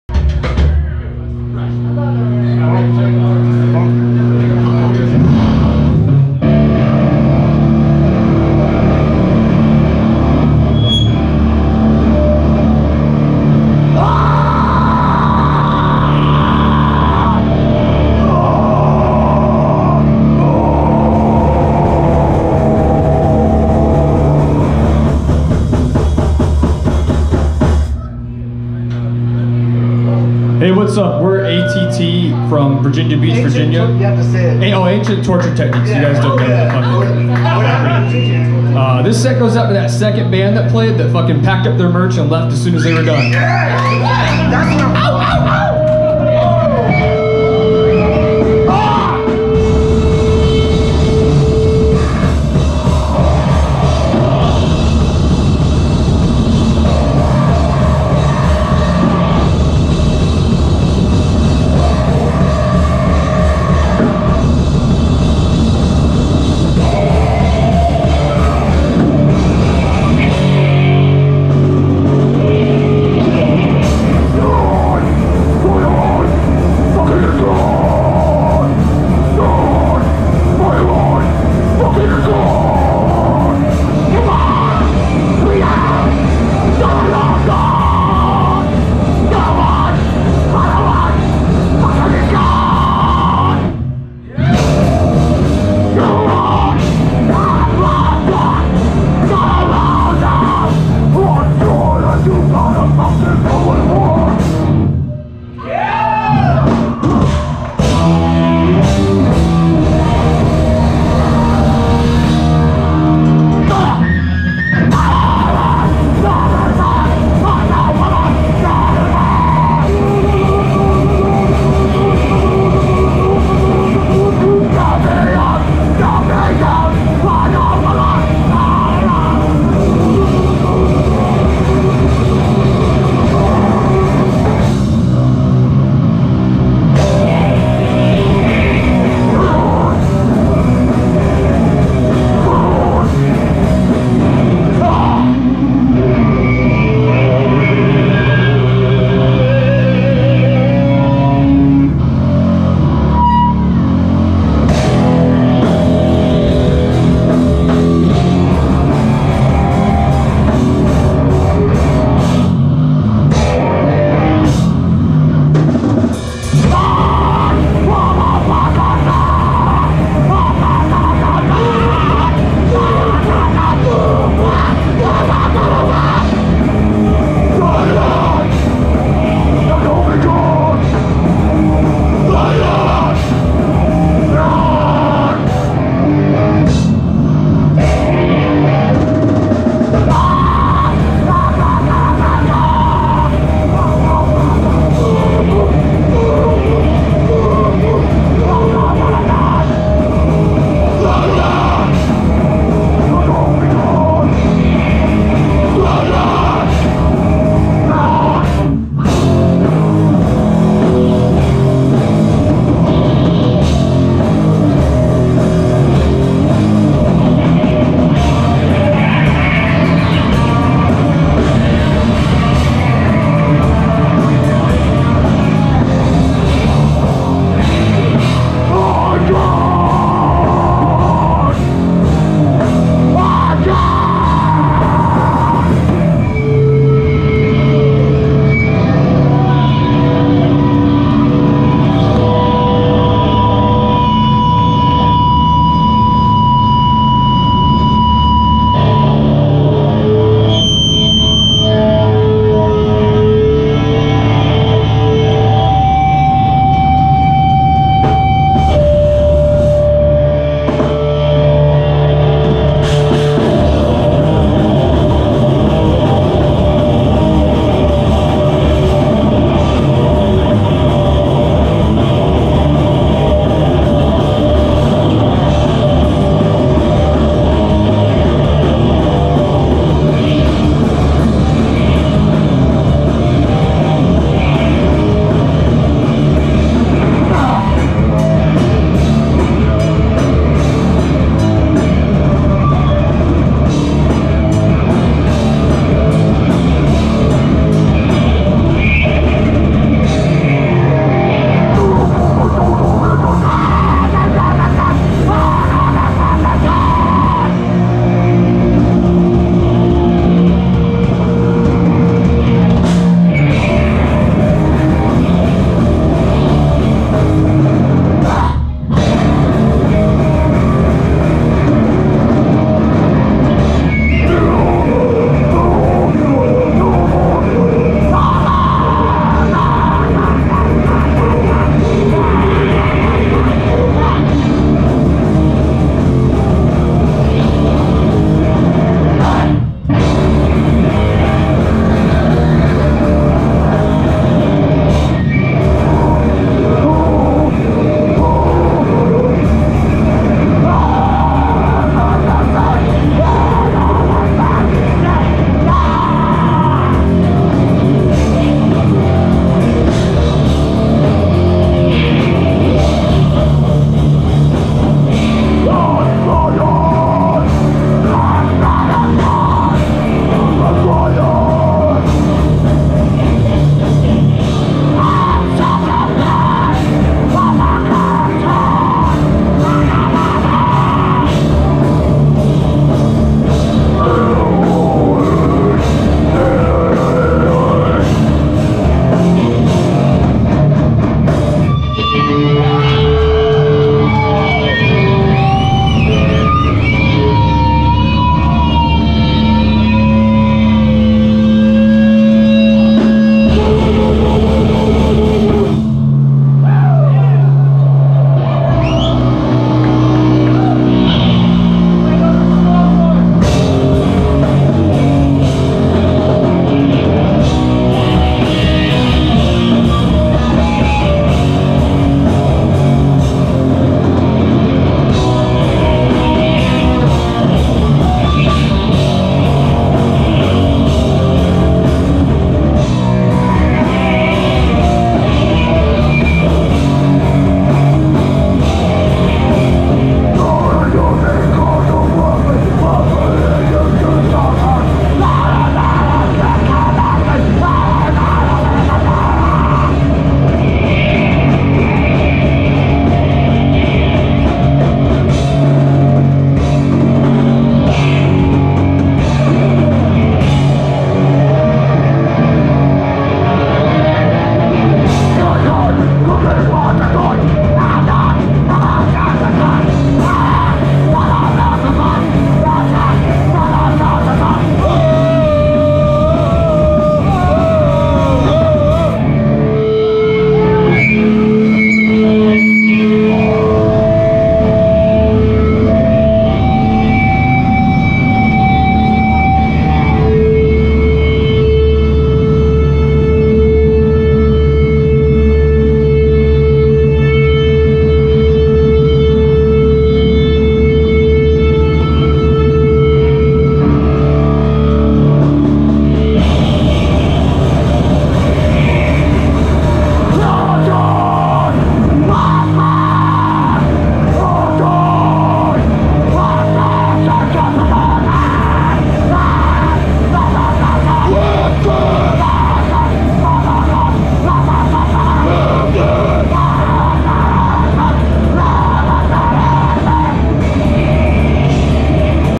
ANCIENT TORTURE TECHNIQUES 352 FEST 4.21.2019 GAINESVILLE FL PART 1 GRINDCORE POWER VIOLENCE
ANCIENT TORTURE TECHNIQUES AT 352 FEST ON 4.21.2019 IN  GAINESVILLE FL PART 1